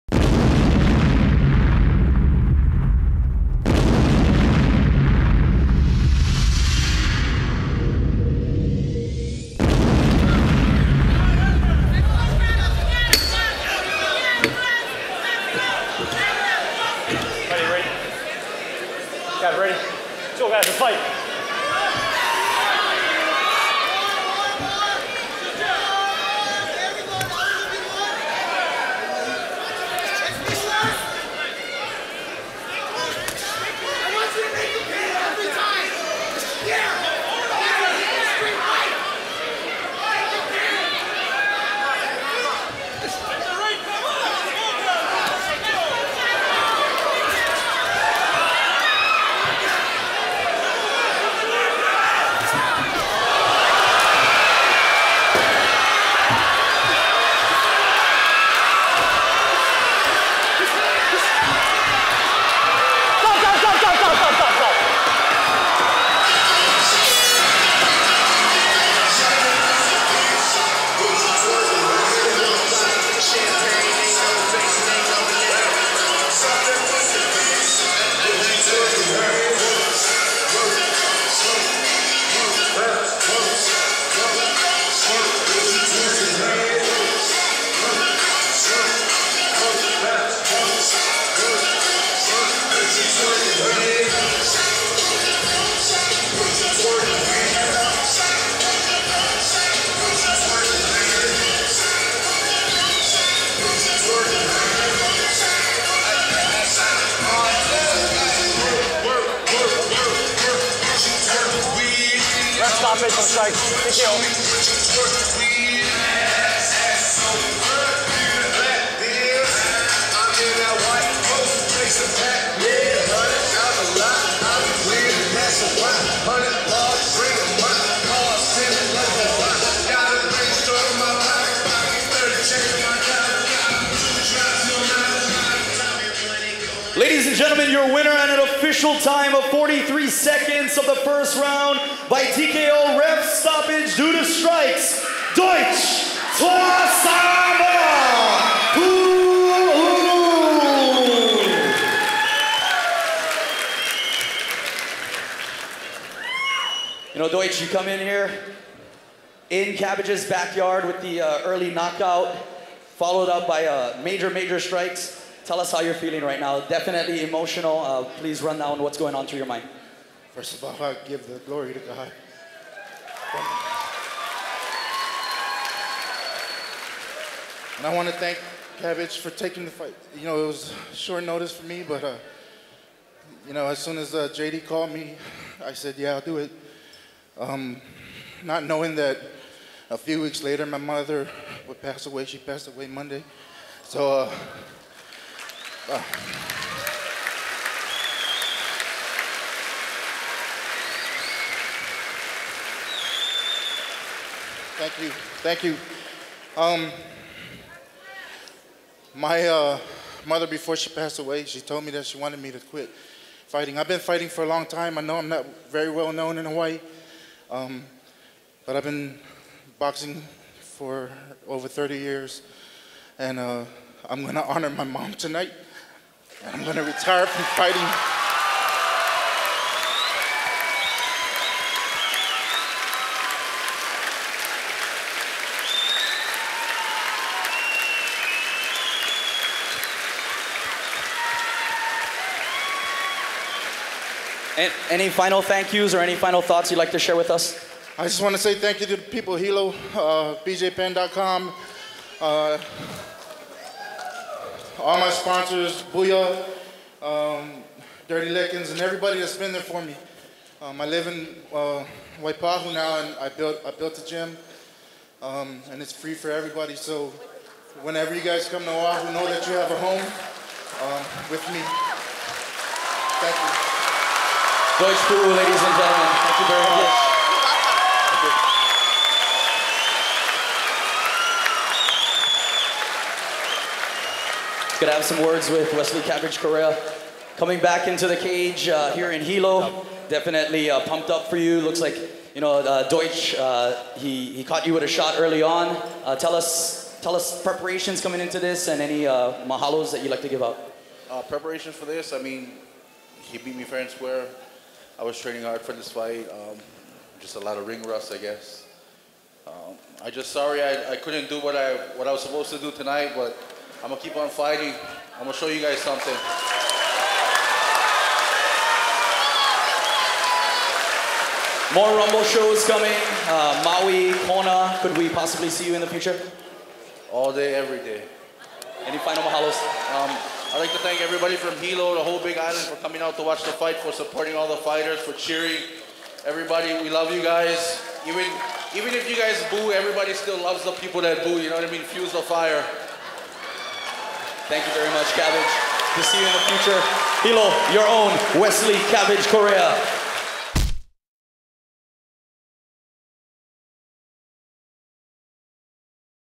Just Scrap Fight 11 - Deutsch Pu`u VS Cabbage Correira
RWE & BJPENN.com present Just Scrap 1/19/12 in Hilo at the Civic Auditorium.